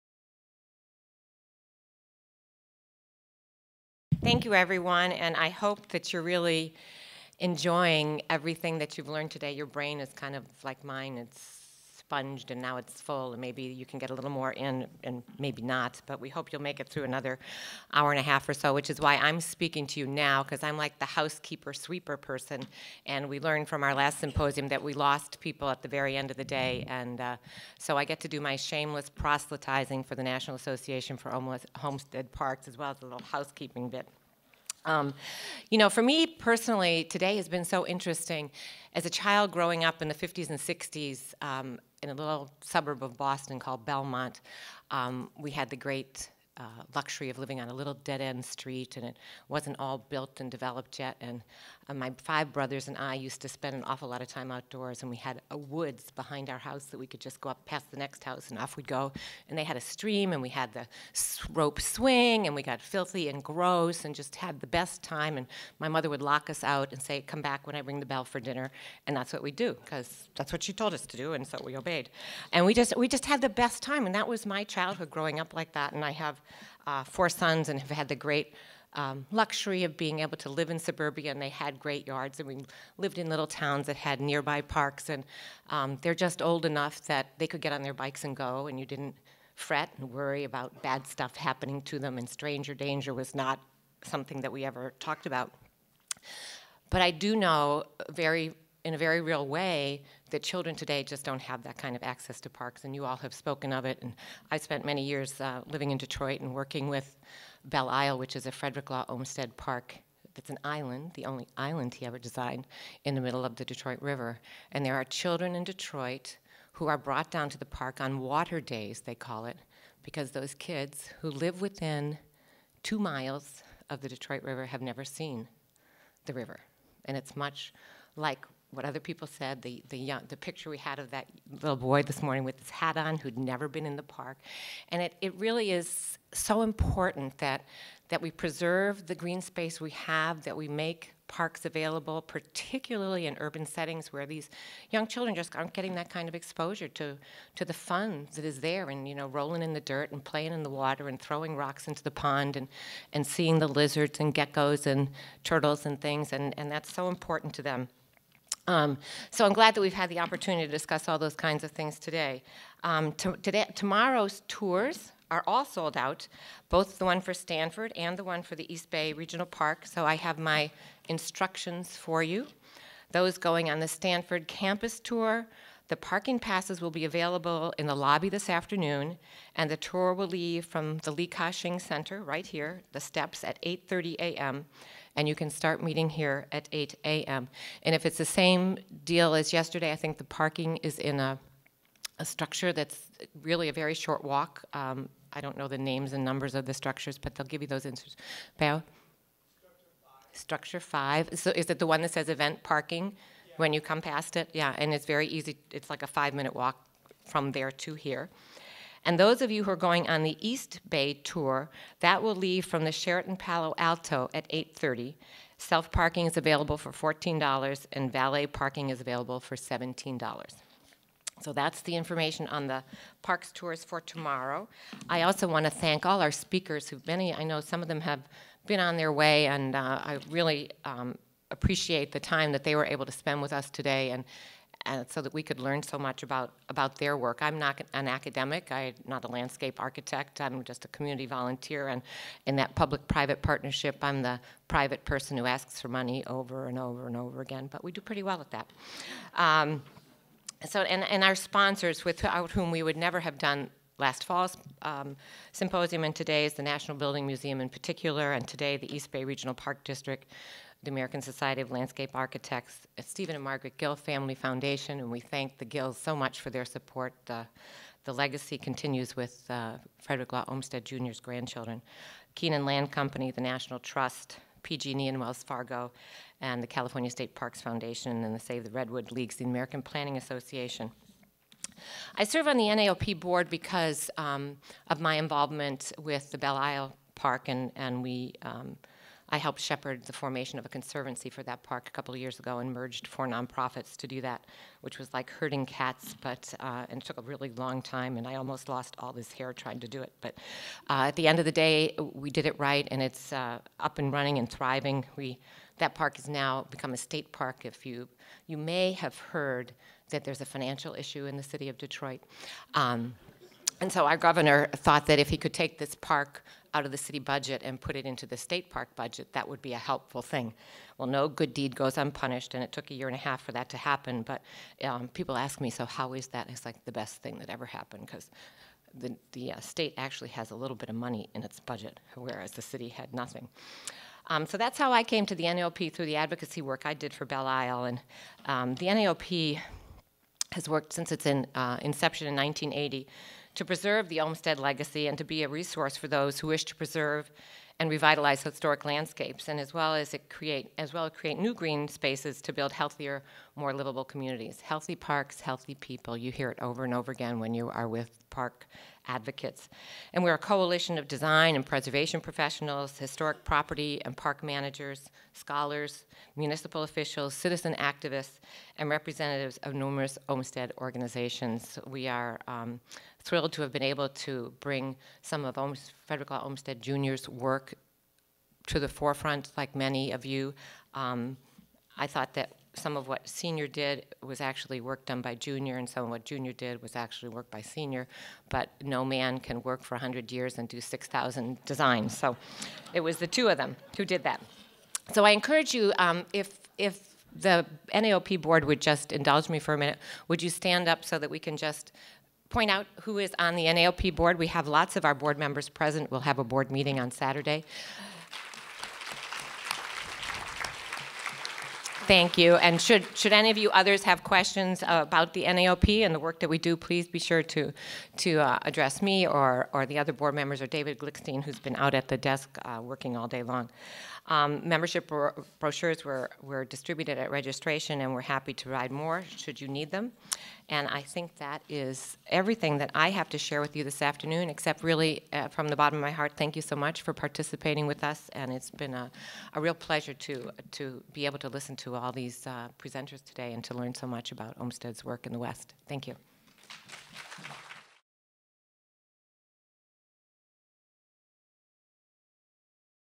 Frederick Law Olmsted Jr: A Vision for the American West: Closing Remarks
Closing Remarks by Sarah Earley, Vice Chair, National Association for Olmsted Parks.

See all videos from Frederick Law Olmsted Jr: A Vision for the American West